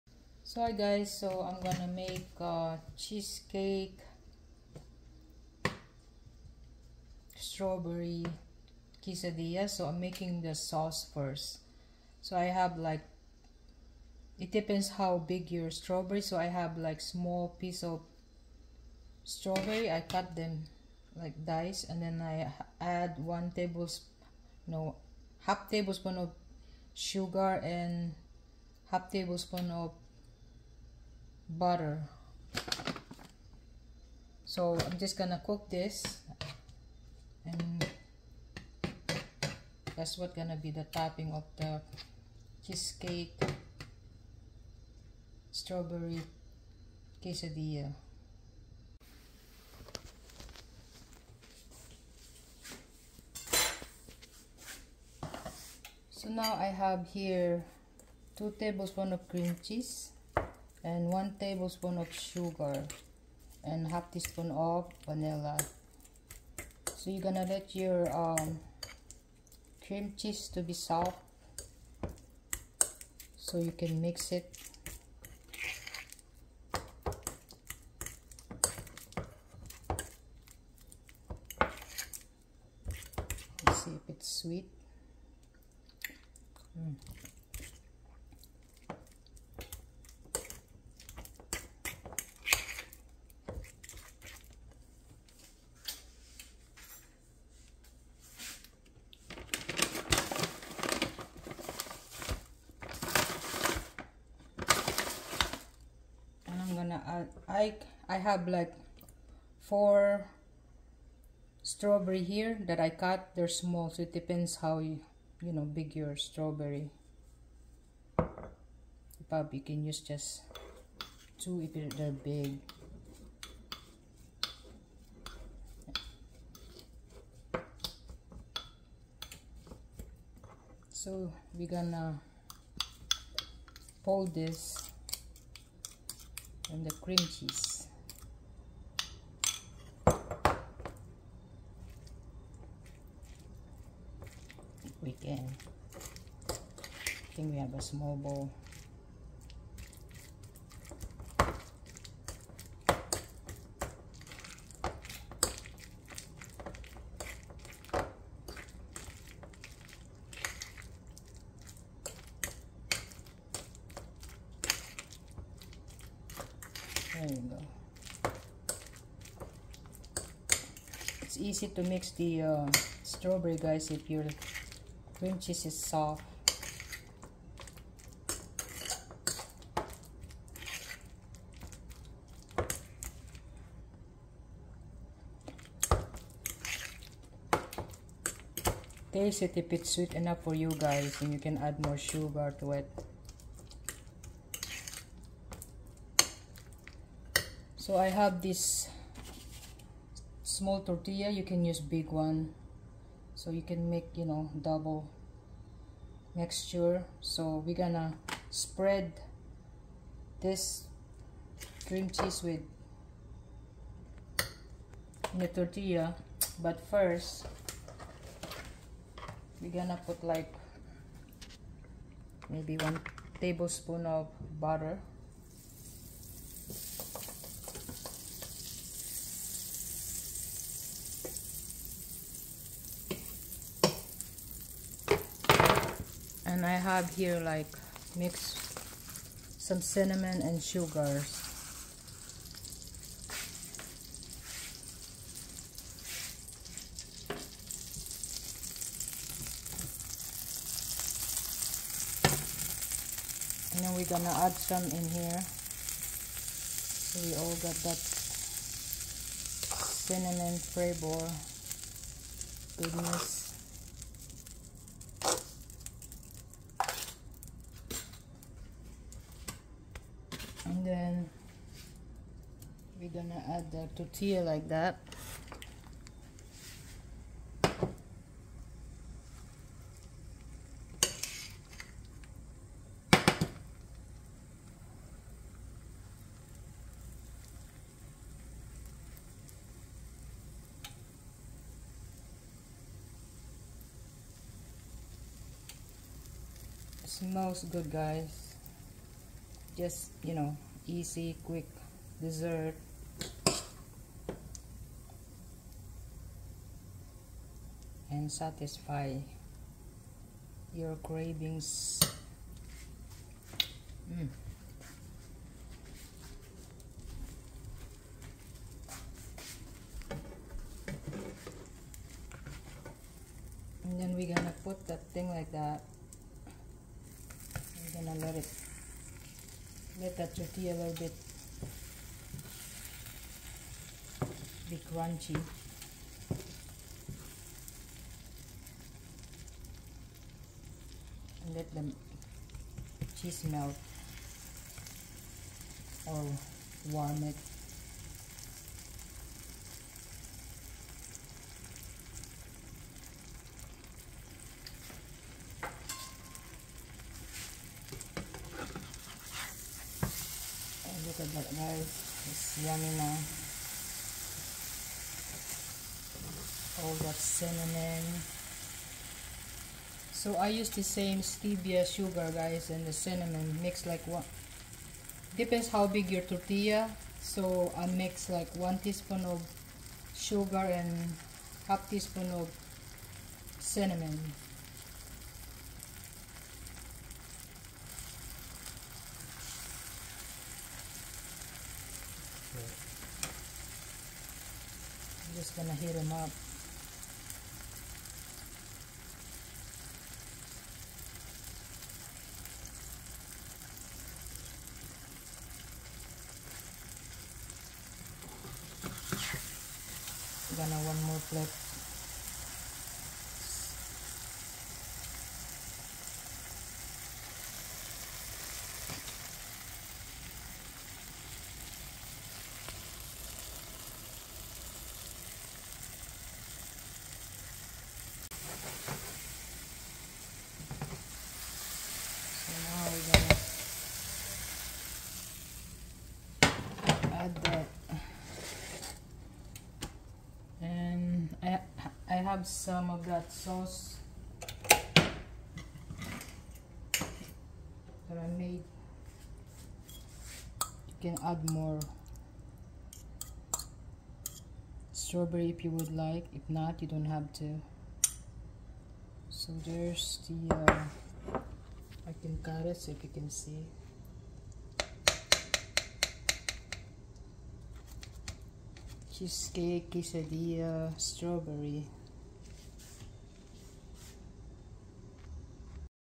Strawberry Cream Cheese quesadilla
Here is a yummy and easy dessert that I love.  Have a great rest of your day.